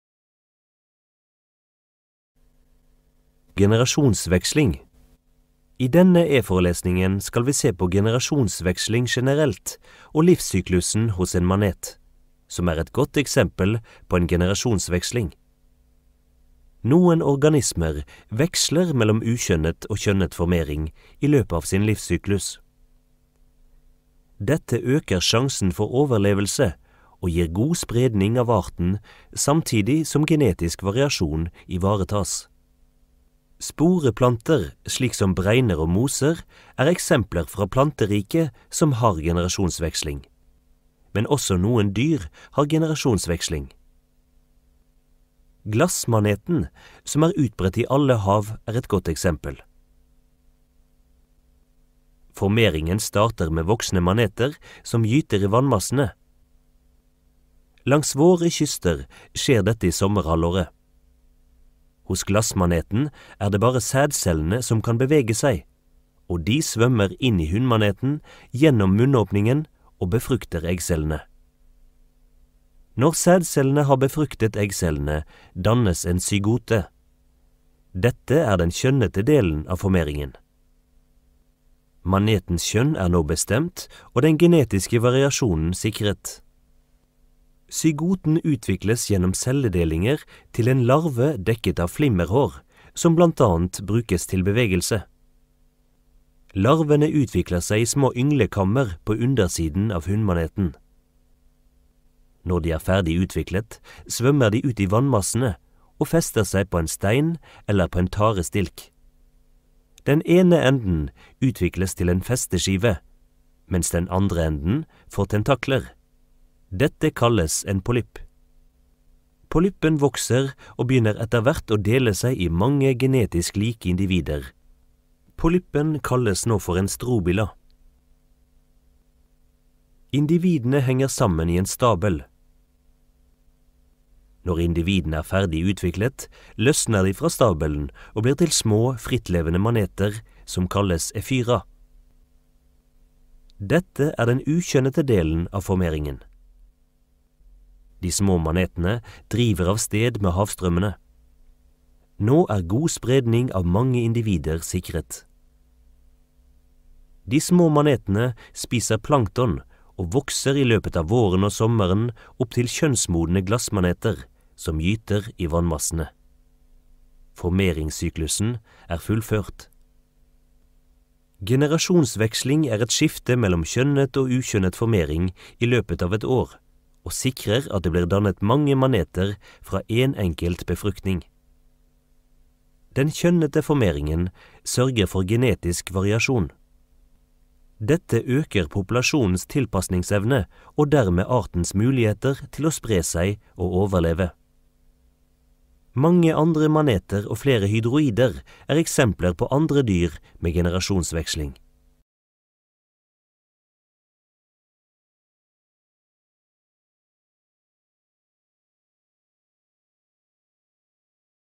Generasjonsveksling
Generasjonsveksling er et skifte mellom kjønnet og ukjønnet formering, og sikrer at det blir dannet mange individer fra en enkelt befruktning.